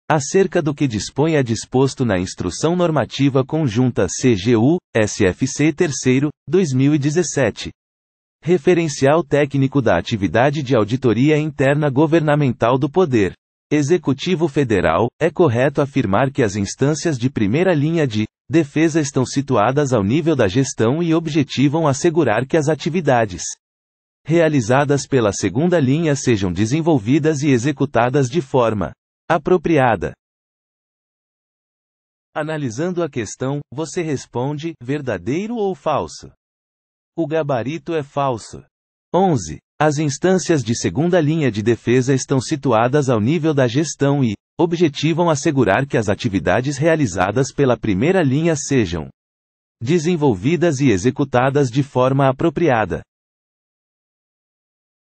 QScon Direito: [Item 11, IN Conjunta CGU/SFC 3°/2017] Texto de Lei
Item 11, IN Conjunta CGU/SFC 3°/2017 | Texto de Lei

Acerca do que dispõe a disposto na Instrução Normativa Conjunta CGU/SFC terceiro. /2017. (Referencial Técnico da Atividade de Auditoria Interna Governamental do Poder. Executivo Federal), é correto afirmar que as instâncias de primeira linha de. defesa estão situadas ao nível da gestão e objetivam assegurar que as atividades. realizadas pela segunda linha sejam desenvolvidas e executadas de forma. apropriada.

Gabarito: Falso